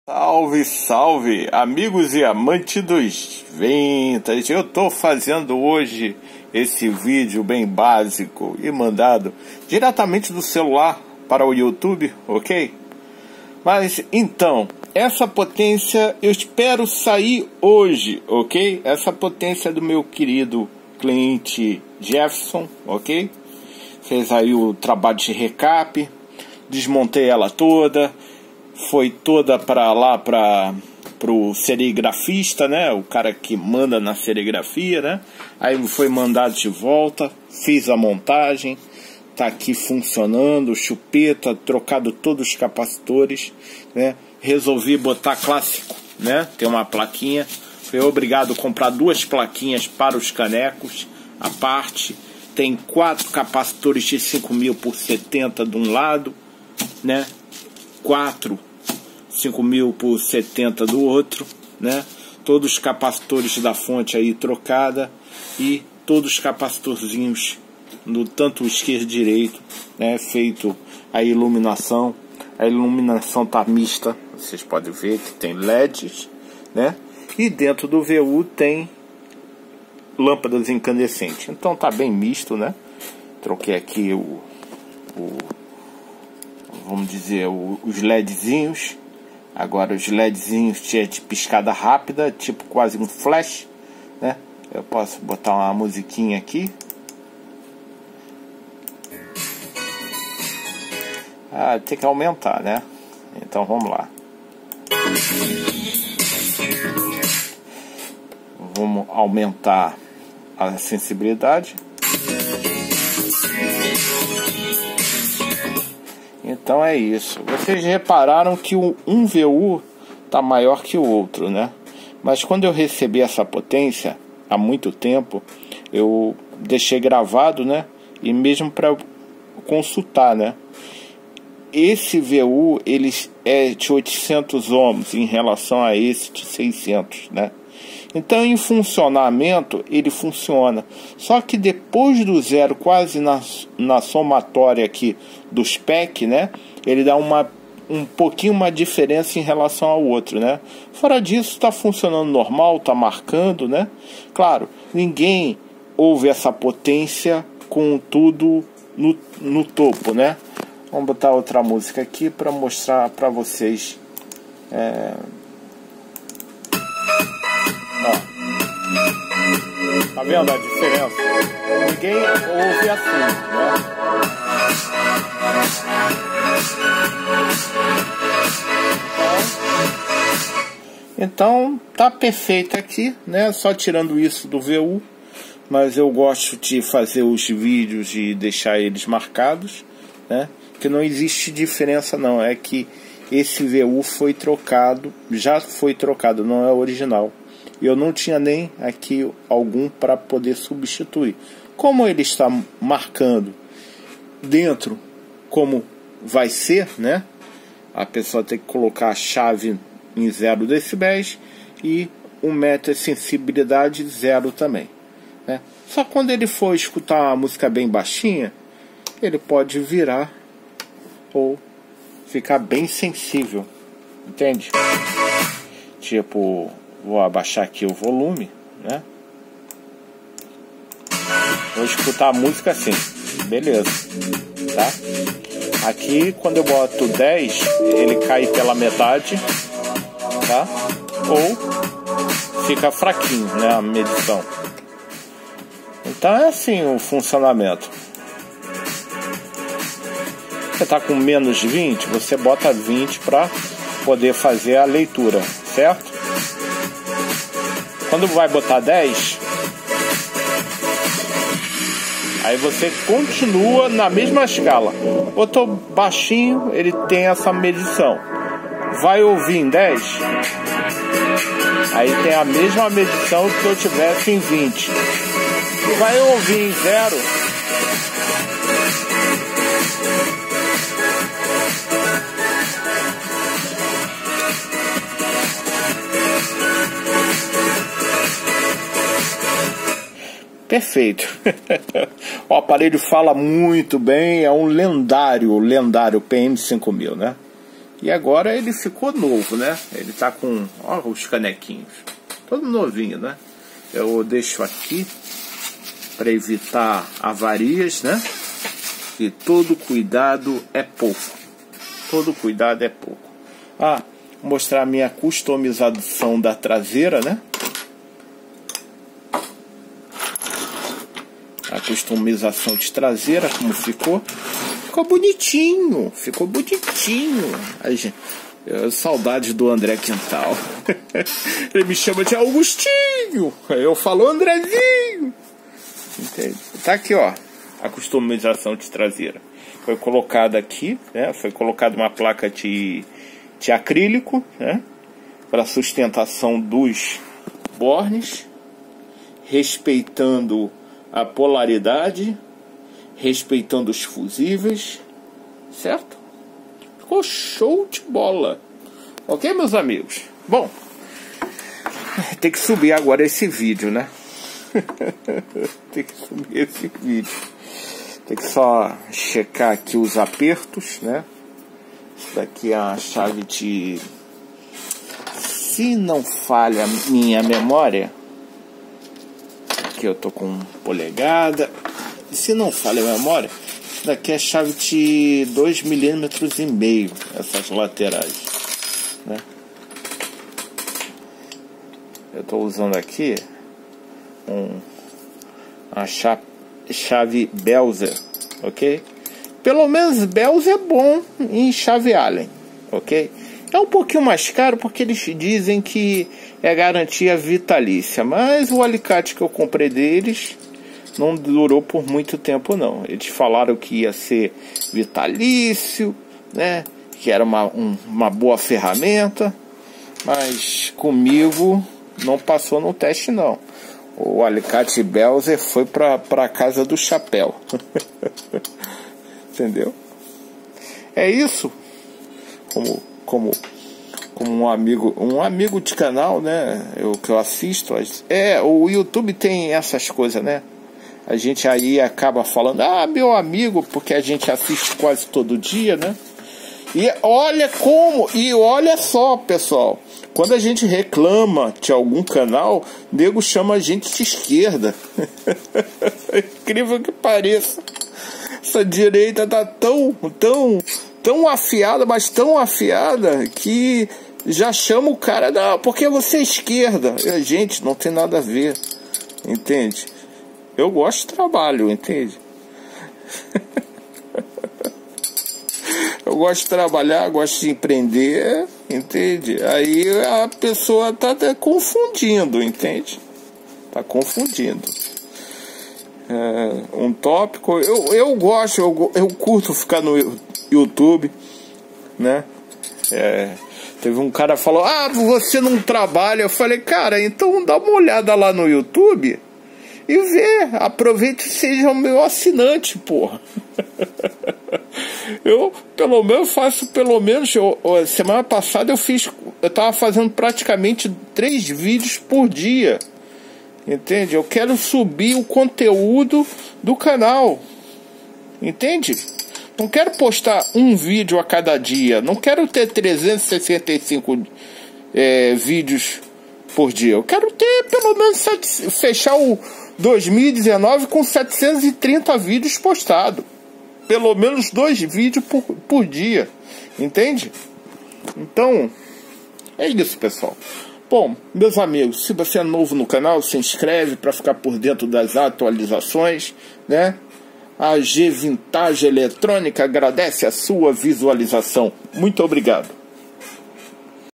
O Lendario PM5000 Aguardando o Cliente Jerfeson pega-lo
E Meus amigos mais um lendário todo Reformado e Customizado e Recapeado na AG Vintage Eletrônica

(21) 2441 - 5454 Ramal 20